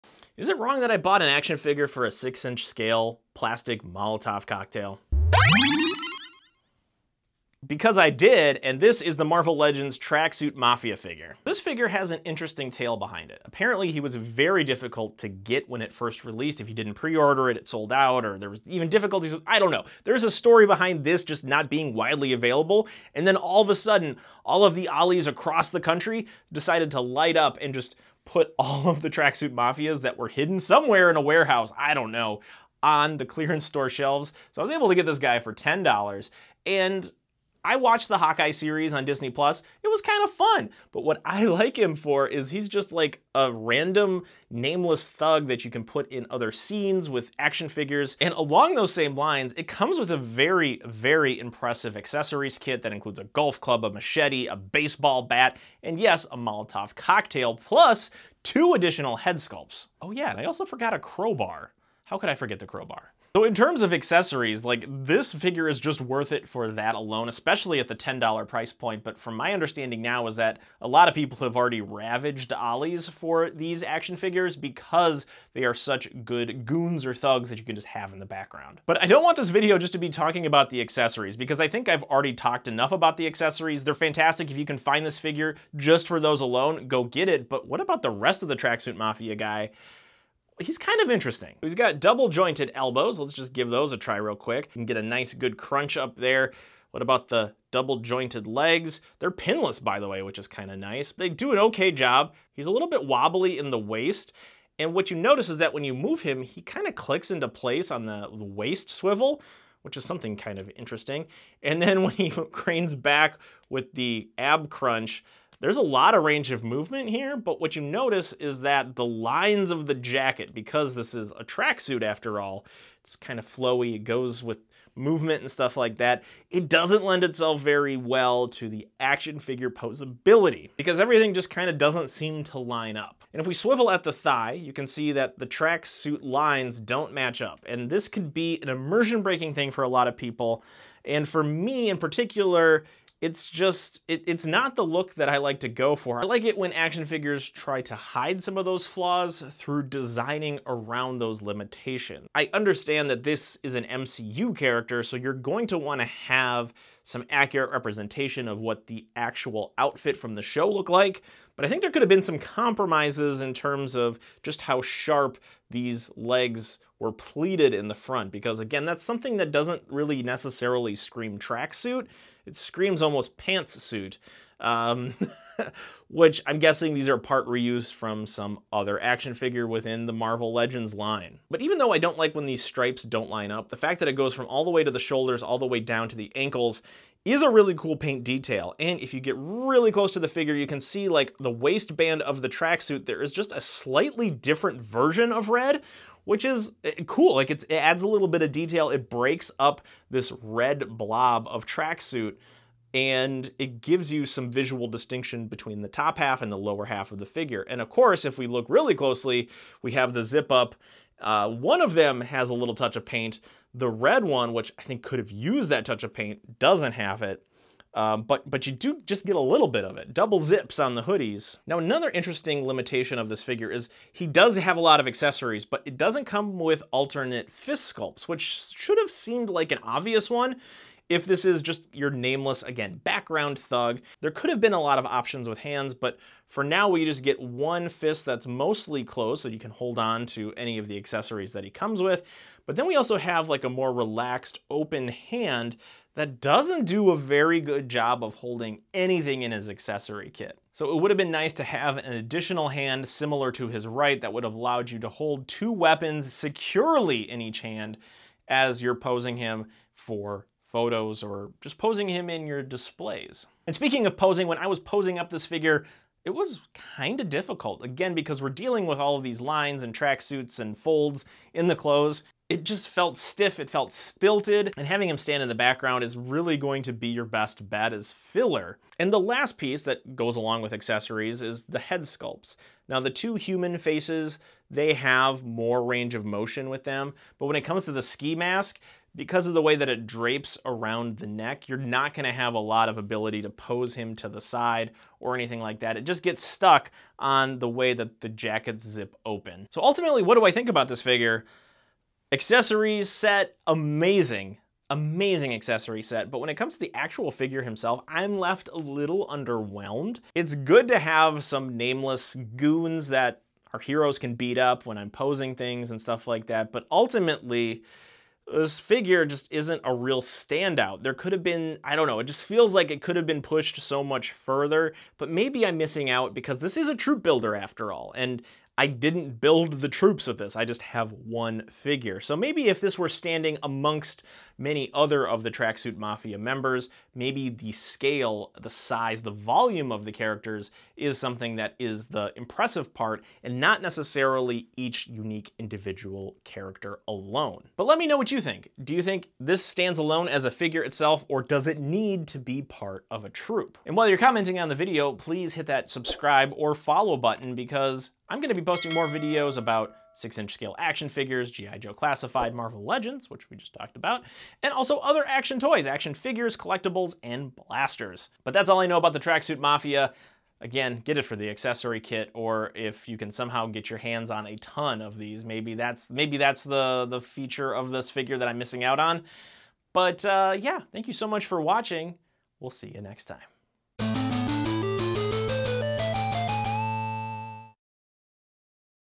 3 Figures for the PRICE OF ONE - Marvel Legends Track Suit Mafia
The Track Suit Mafia from Marvel Legends is 3 characters in one. From the Hawkeye Disney+ show, this figure has a TON of accessories and multiple head sculpts.

This was a hard to find figure, but now they are popping up at Ollie's Discount stores. Are they worth finding? Watch on to find out.